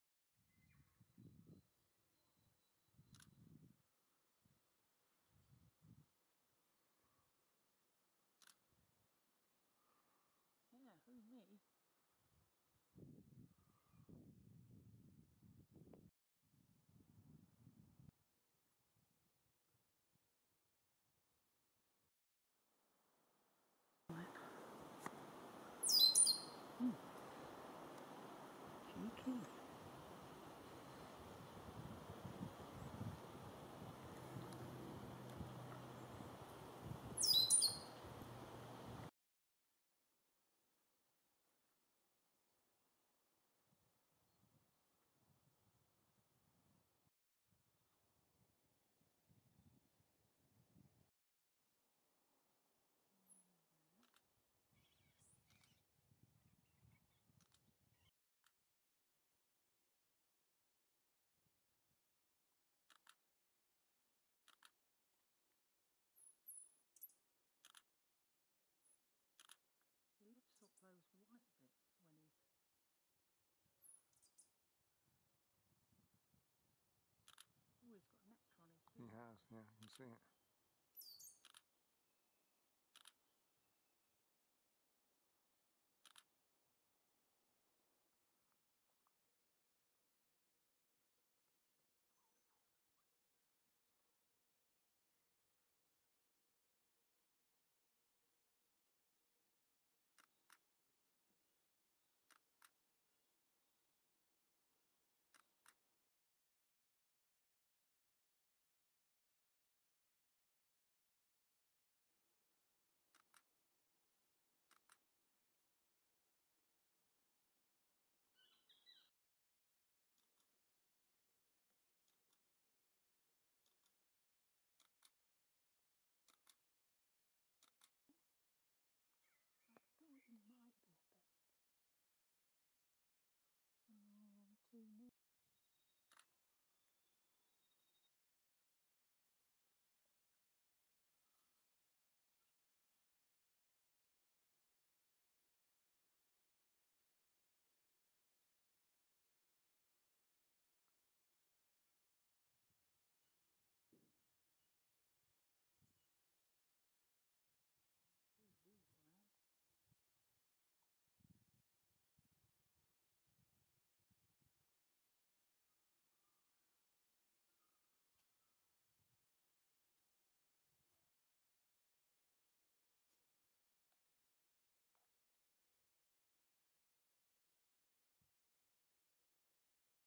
Bushy Park Bird Sanctuary
Filmed at Bushy Park Bird Sanctuary, North Island, New Zealand, 25 October 2019.